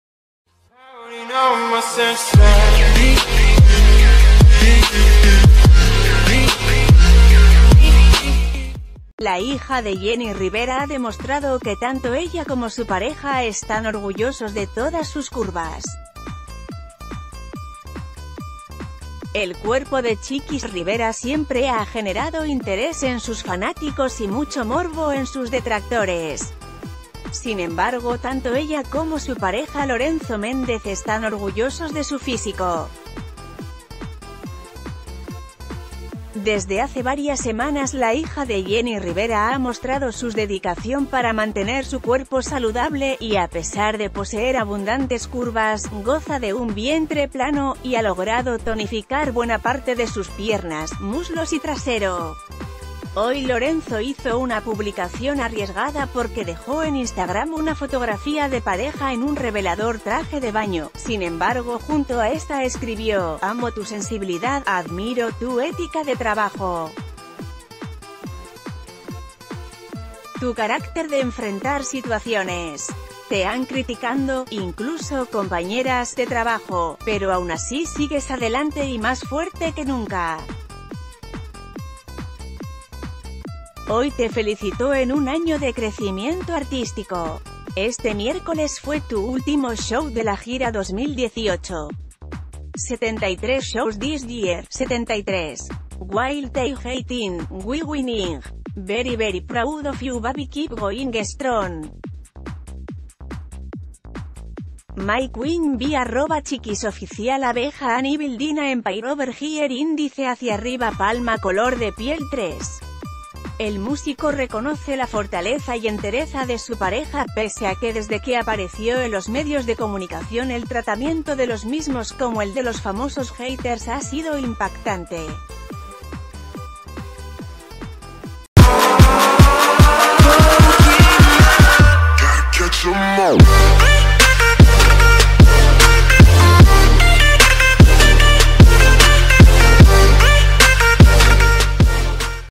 Lorenzo Méndez presume a su novia, Chiquis Rivera, en un impresionante traje de baño
La hija de Jenni Rivera ha demostrado que tanto ella como su pareja están orgullosos de todas sus curvas... El cuerpo de Chiquis Rivera siempre ha generado interés en sus fanáticos y mucho morbo en sus detractores. Sin embargo tanto ella como su pareja Lorenzo Méndez están orgullosos de su físico. D...